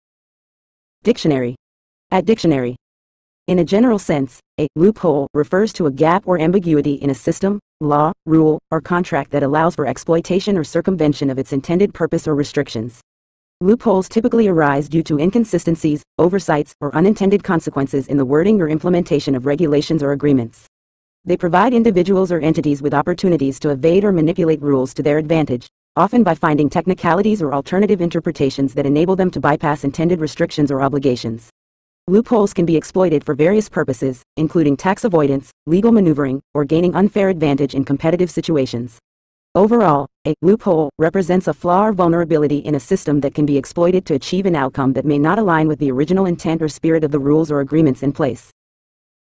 Loophole Meaning In English
In a general sense, a "loophole" refers to a gap or ambiguity in a system, law, rule, or contract that allows for exploitation or circumvention of its intended purpose or restrictions. Loopholes typically arise due to inconsistencies, oversights, or unintended consequences in the wording or implementation of regulations or agreements. They provide individuals or entities with opportunities to evade or manipulate rules to their advantage, often by finding technicalities or alternative interpretations that enable them to bypass intended restrictions or obligations. Loopholes can be exploited for various purposes, including tax avoidance, legal maneuvering, or gaining unfair advantage in competitive situations. Overall, a "loophole" represents a flaw or vulnerability in a system that can be exploited to achieve an outcome that may not align with the original intent or spirit of the rules or agreements in place.